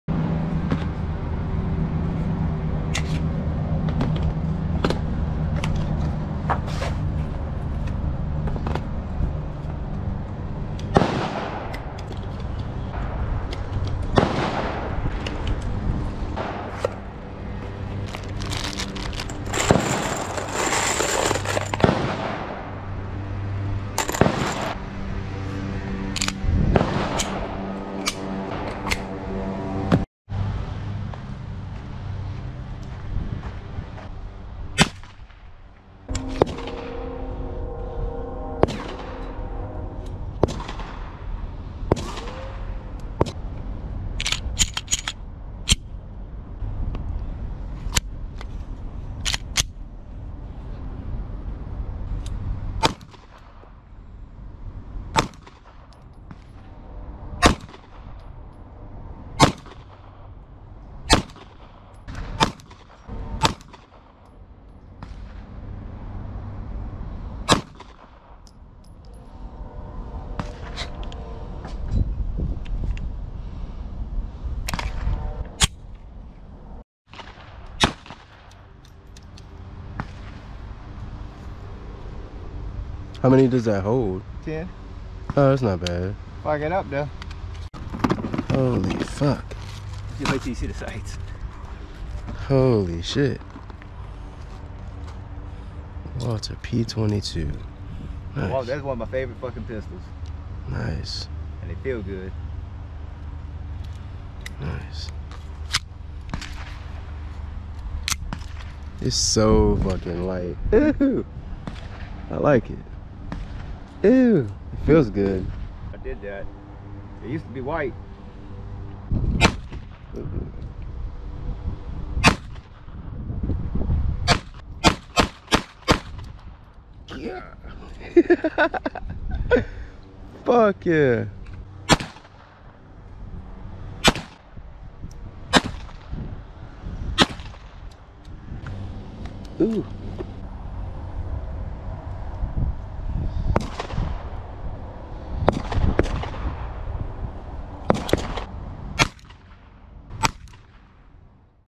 Gopro 9 POV Gun ASMR | Glock 19 & Walther p22
Gopro 9 POV Gun ASMR | Glock 19 @Dylanverduntv A day at the gun range in Louisiana. Glock 19 Gen 4, Walther p22, & more. Shooting with Dylan on a Sunday. I definitely want to upload more firearm content for you guys in the future.